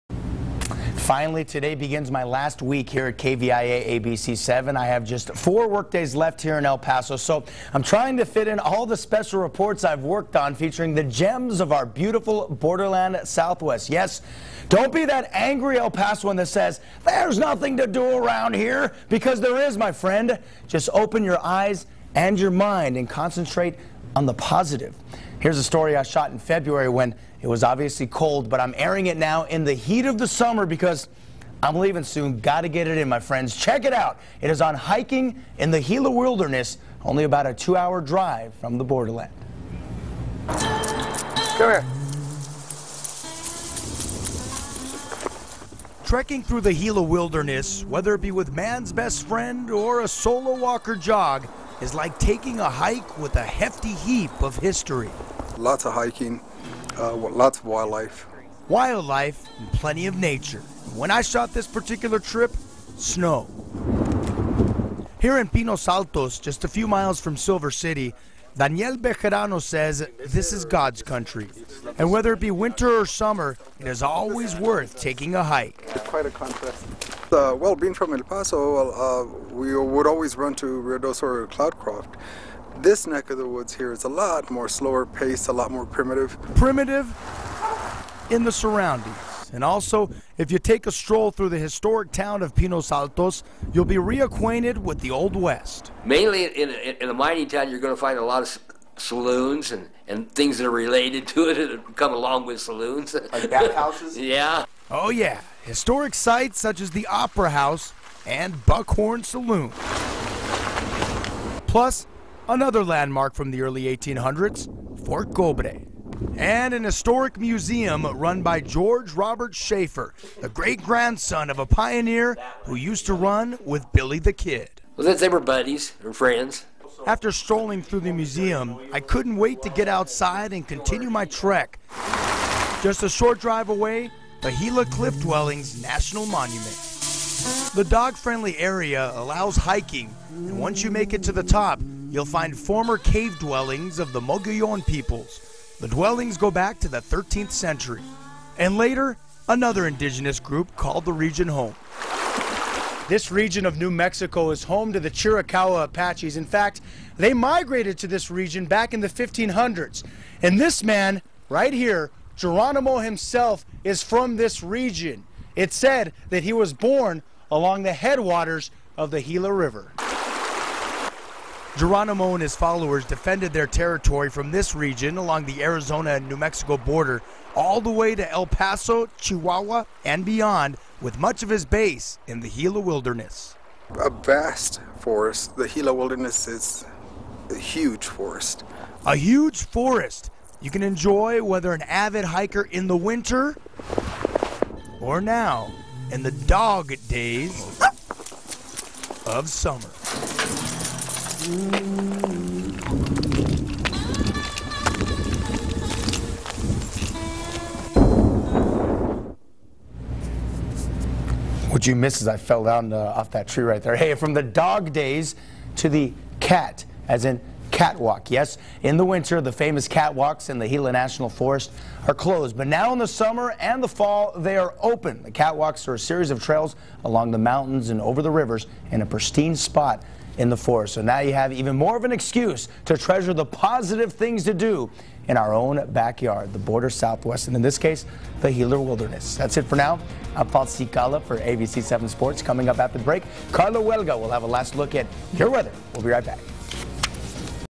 ABC-7's Paul Cicala Reports: Hiking & History at Gila National Forest In New Mexico.
KVIA ABC-7 continues his series of travel and visiting gems in the border southwest.  Cicala hikes through Indian Cliff Dwellings near Pinos Altos, New Mexico and Silver City.